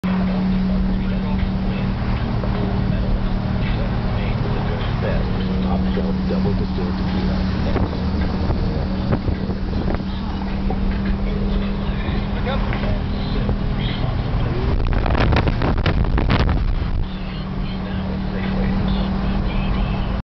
GoldenGate
Driving over the Golden Gate Bridge in October 2006